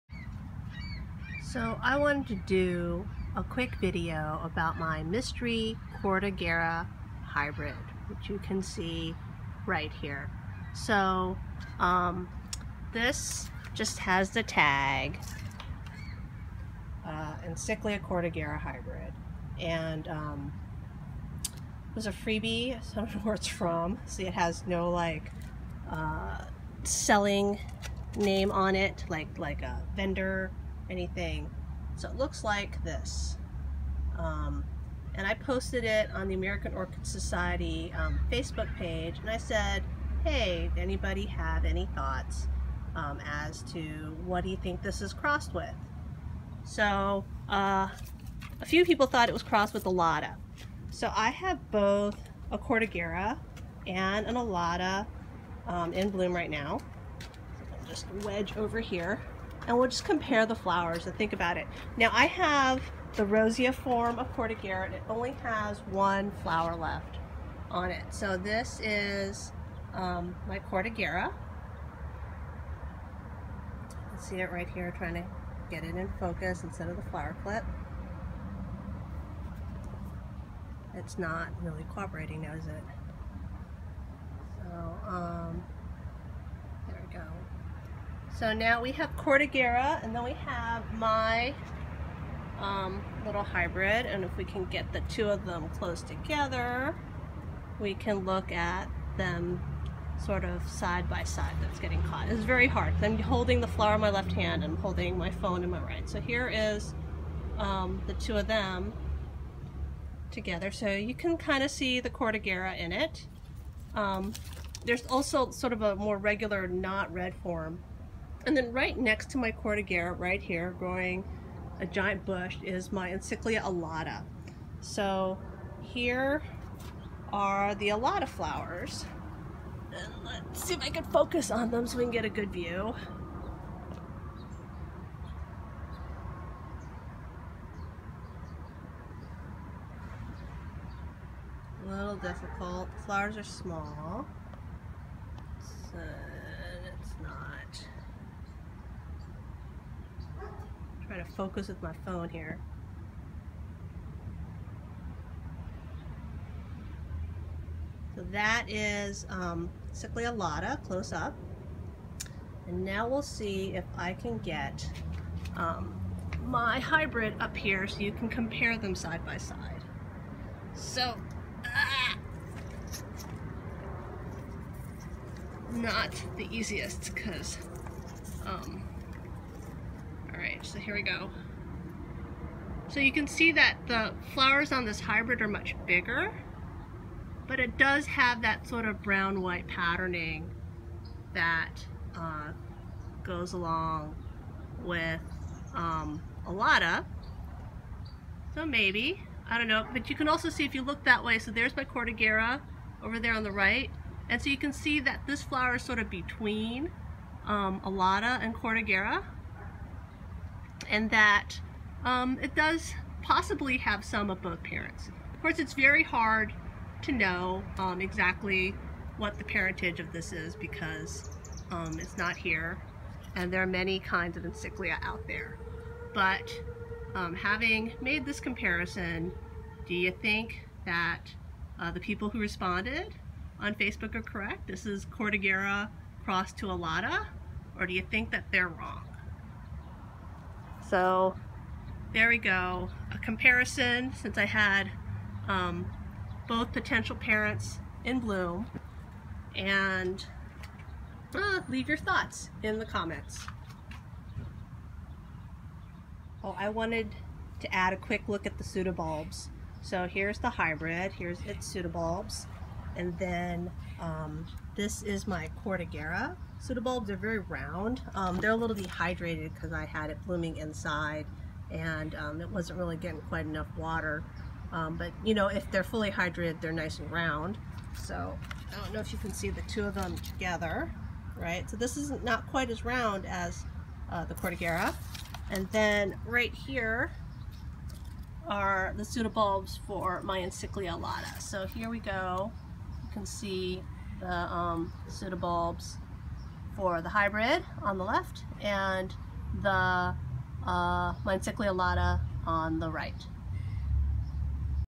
Mystery Encyclia cordigera hybrid parentage
Thoughts on the parentage of an orchid labeled only as Encyclia cordigera hybrid. As I have two potential parents currently blooming, I compare it to Encyclia cordigera and Encyclia alata.